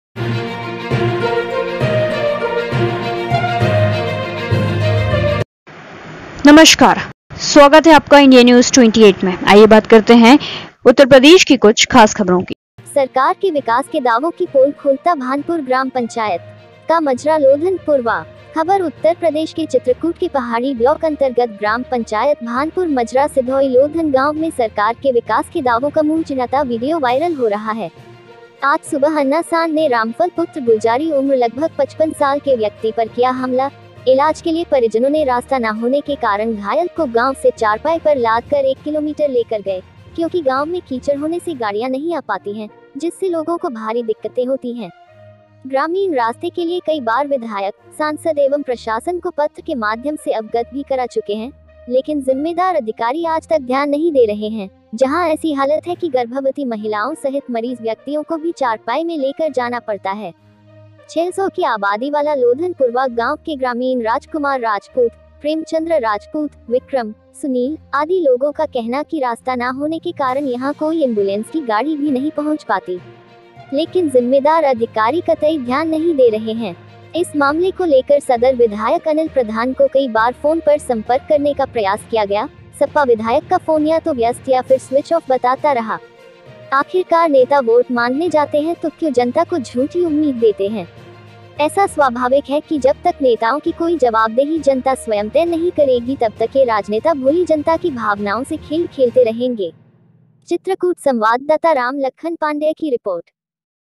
सरकार के विकास के दावों की पोल खोलता भानपुर ग्राम पंचायत
Official